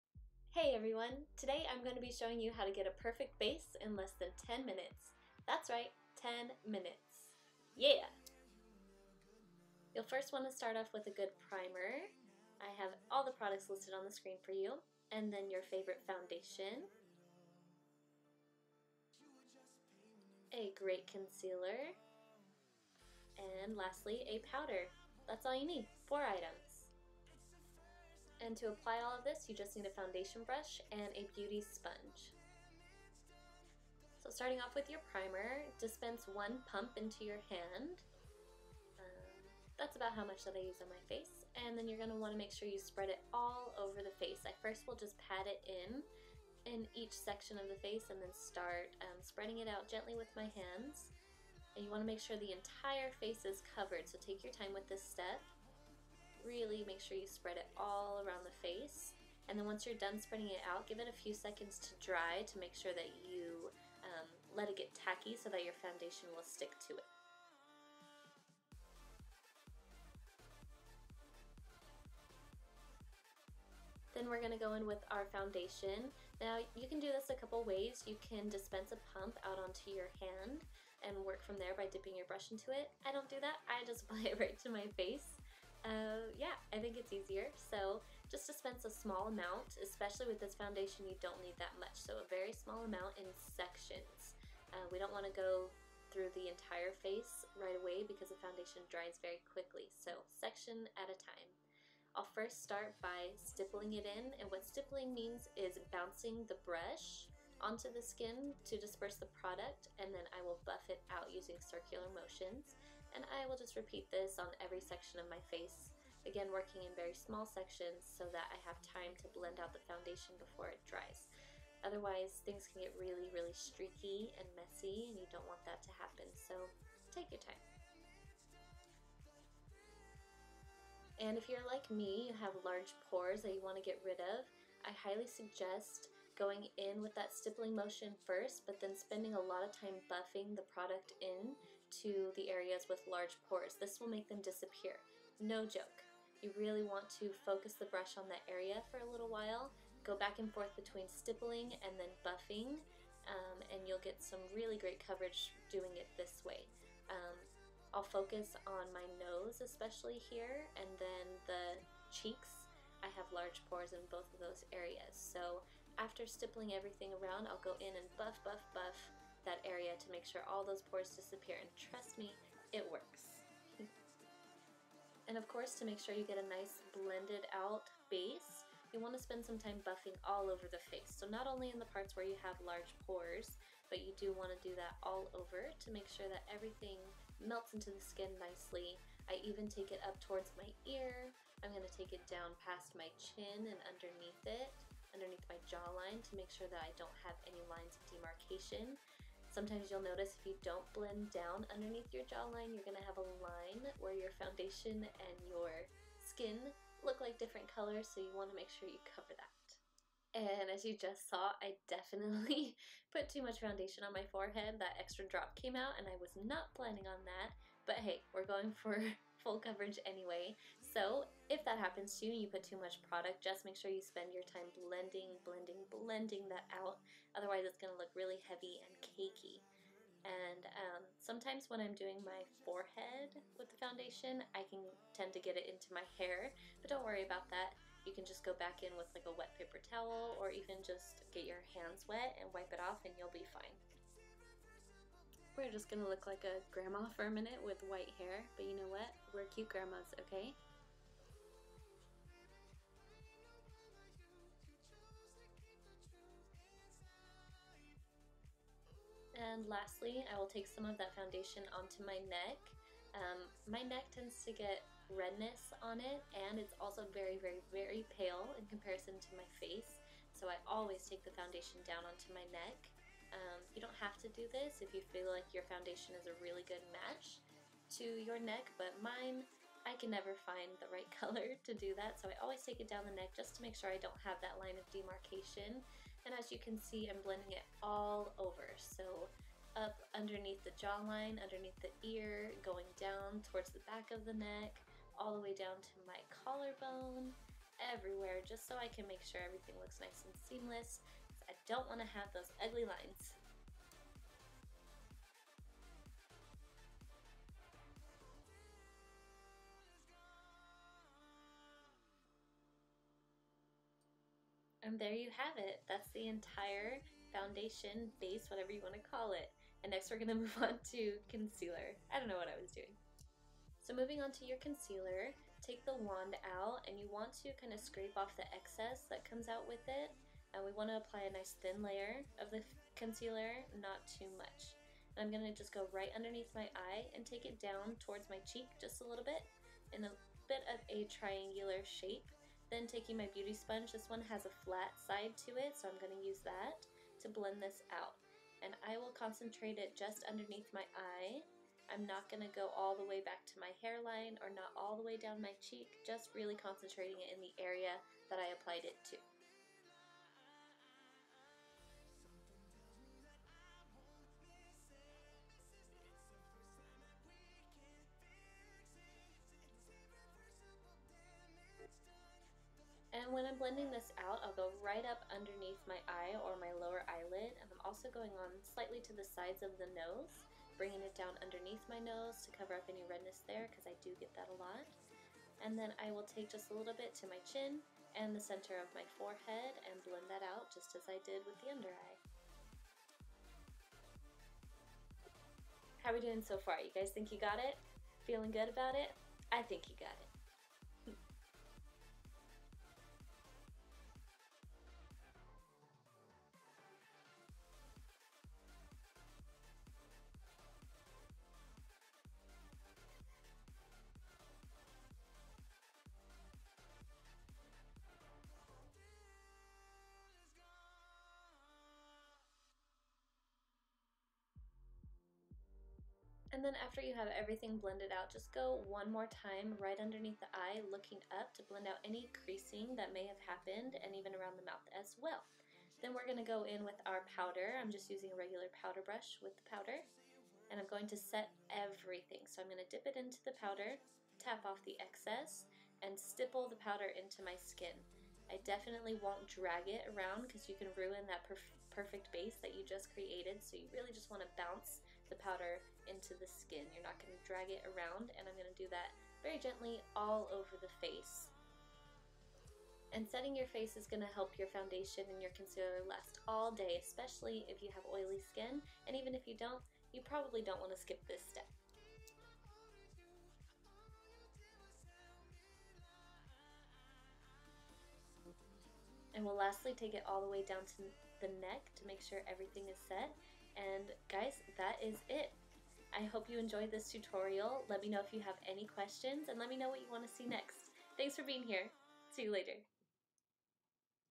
PERFECT BASE in less than 10 minutes! | Stephanie Ashcroft
Learn how to get the perfect base with 4 items in under 10 minutes! I am working on a few more videos just like this to help you learn how to do pretty much ALL beauty basics in less than 10 minutes. I hope you like the concept! See you again soon.

PRODUCTS MENTIONED